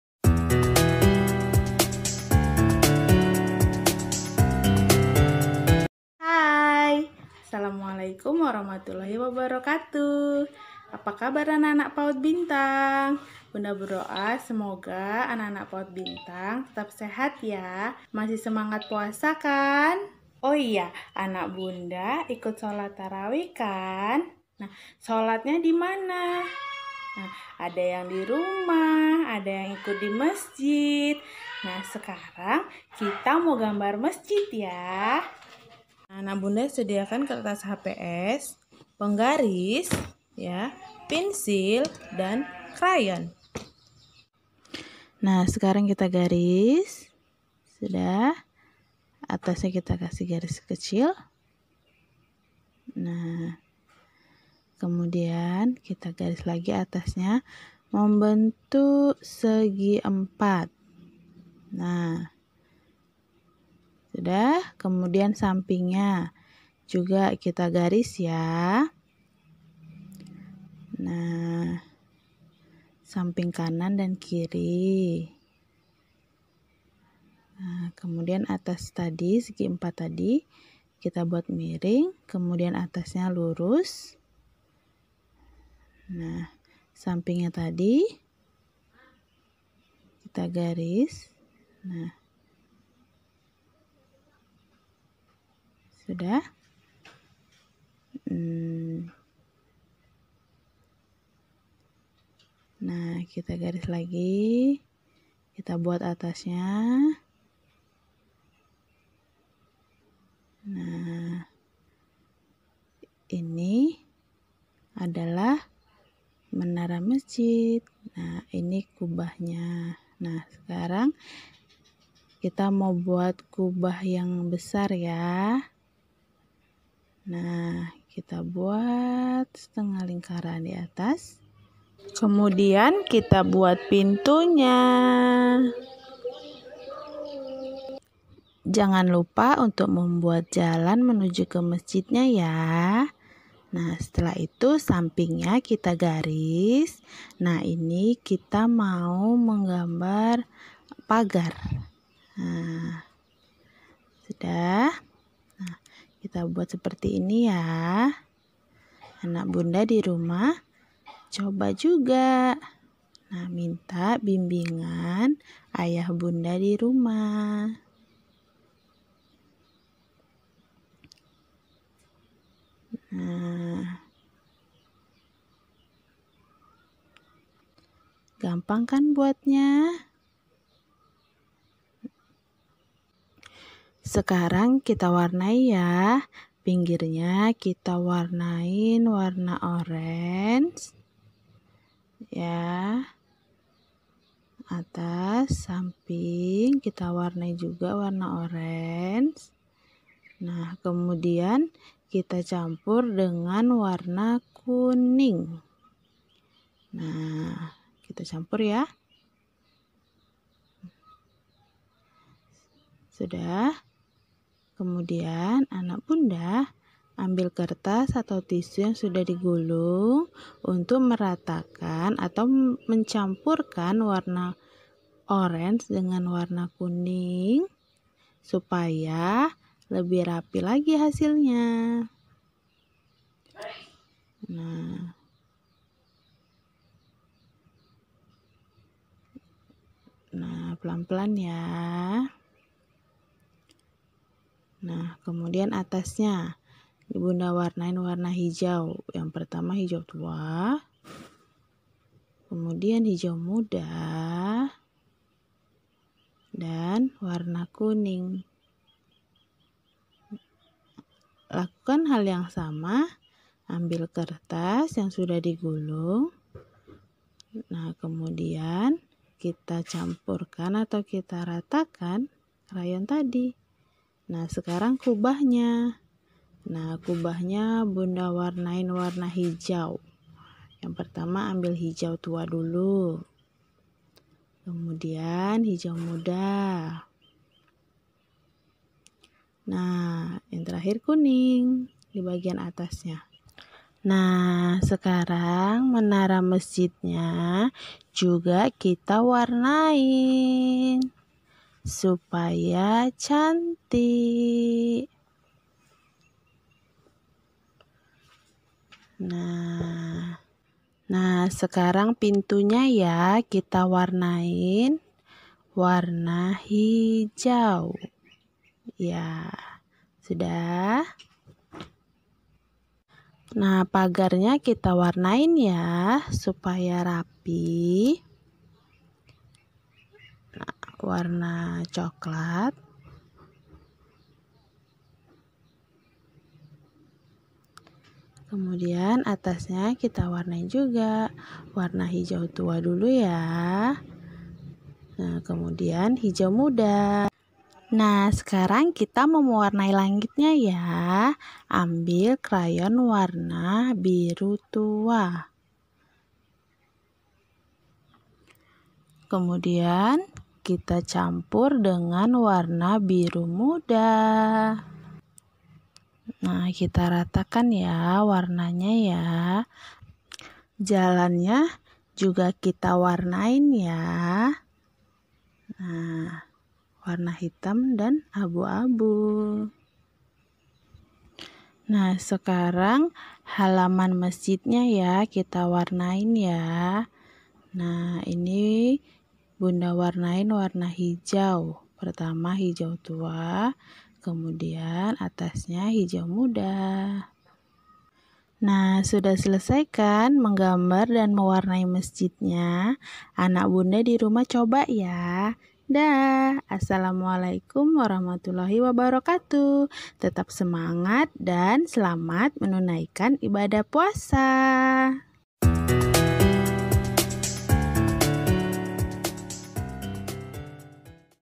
Belajar Daring PAUD Bintang Deli Serdang : Menggambar Mesjid
Pembelajaran dalam jaringan (daring) PAUD Bintang dalam masa lockdown mencegah penyebaran vurus covid 19.

Semoga kita senantiasa sehat, bahagia menjalani aktivitas full di rumah membersamai anak - anak. Insya Allah anak akan tumbuh dan berkembang secara baik dengan adanya kita orang tua di sampingnya.

⭐Salam PAUD BINTANG⭐
Muslim Cerdas, Unggul, Mandiri dan Berakhlakul Karimah